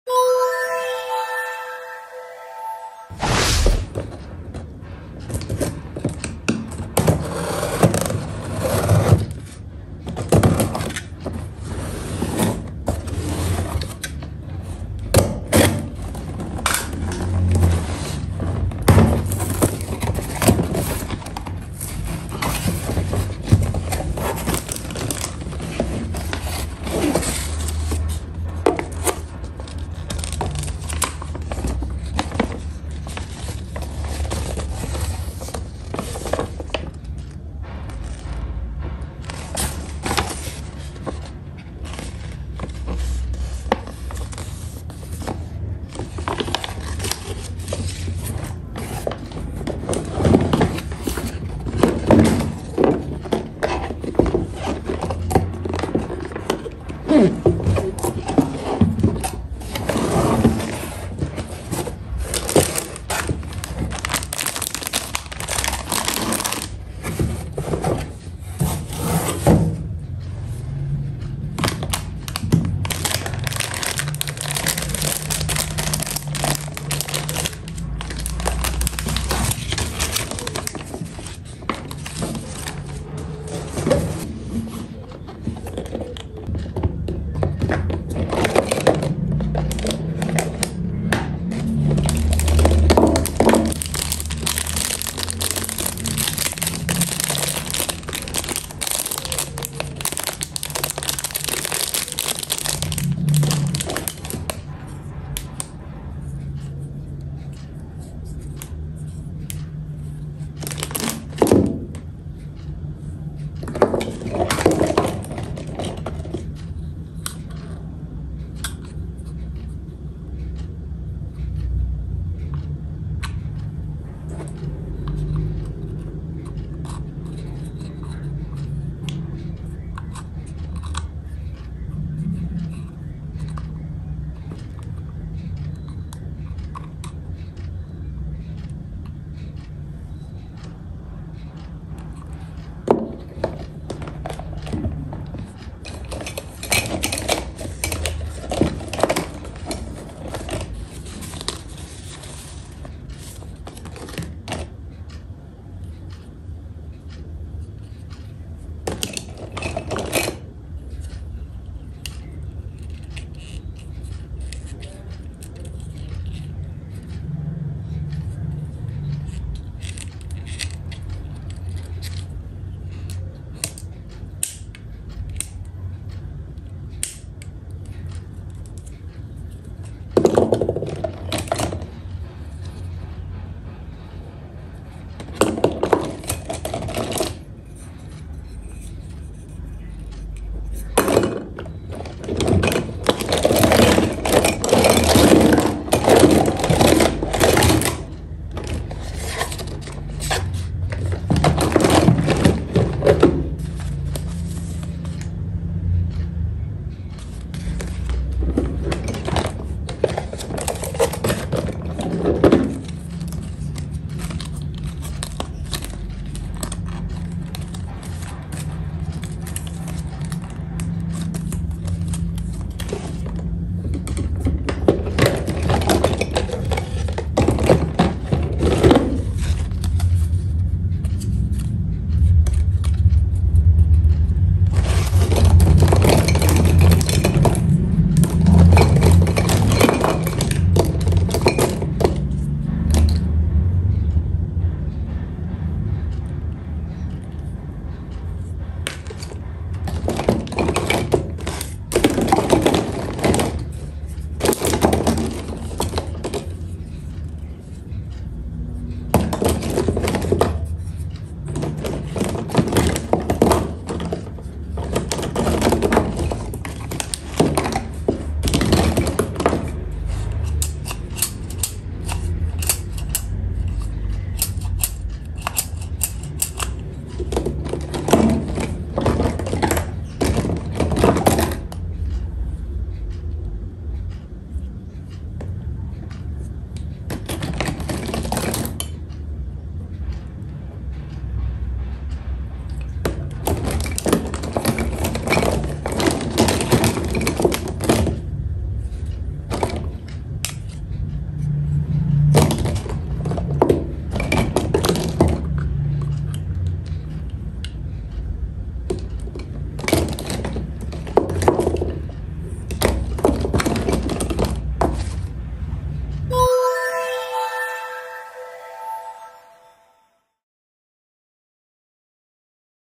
Unboxing Set Mainan Dokter Dokteran
Unboxing Set Mainan Dokter Dokteran. Di dalamnya terdapat bermacam macam alat alat kedokteran. Mainan kreatif yang bisa menambah pengetahuan dan memperluas wawasan. Sangat tepat bila anak anak usia dini dikenalkan pada macam macam mainan ini.
Terima kasih.